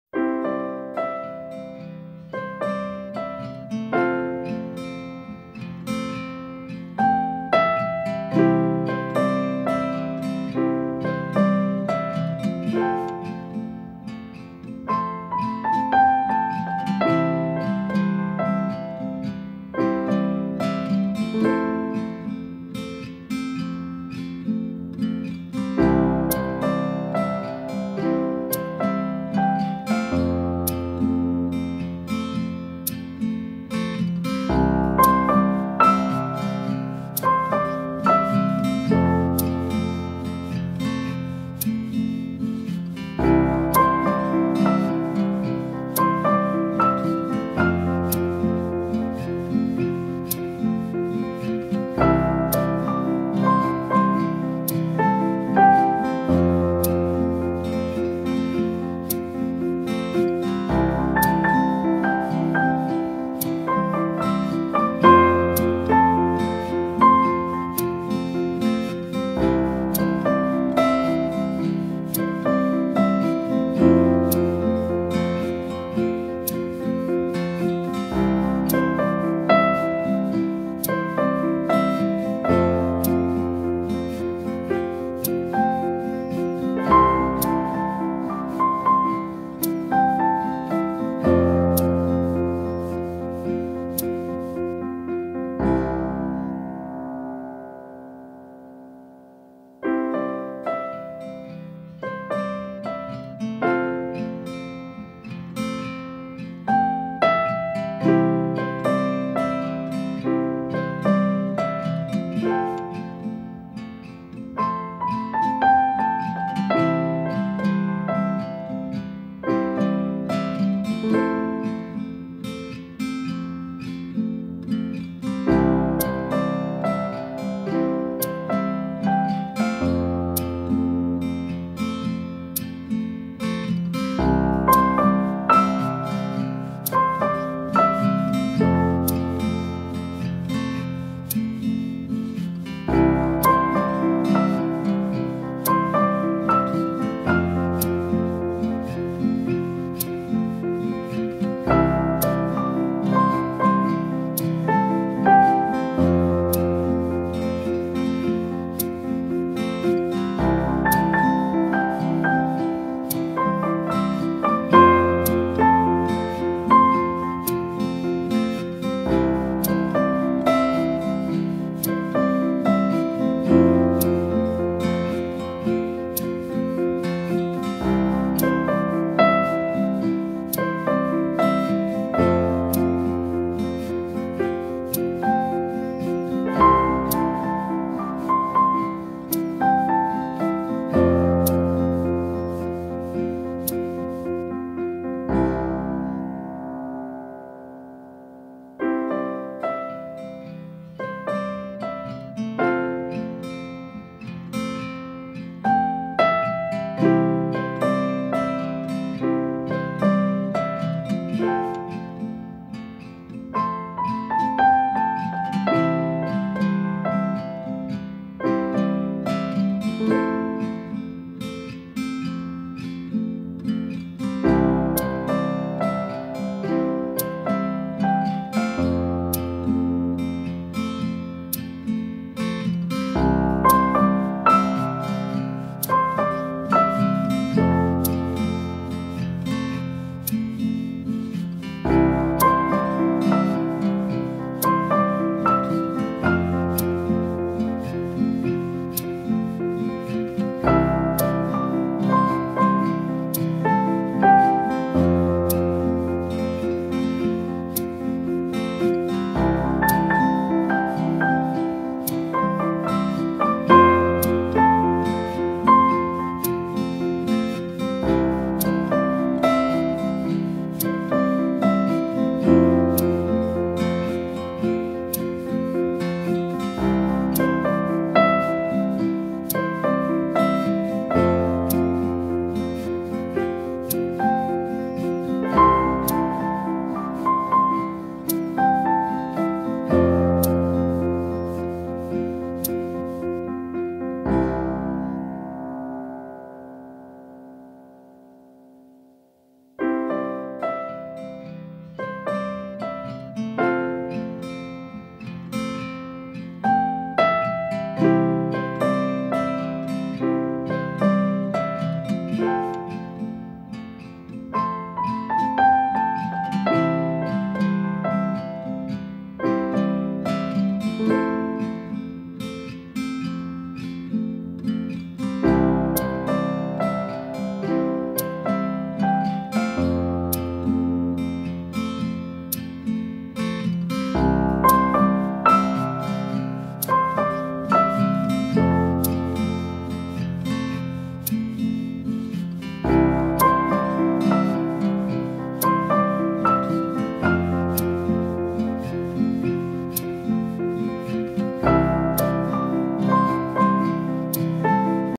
How To Draw A Cute Baby Girl Step By Step //@Amusingartanddrawing
From this video you can learn how to draw a beautiful girl easily.  You can tell how the video went by writing in the comment box.

How to draw a baby girl.Girl drawing. Step by step drawing. Amusing art. Easy drawing.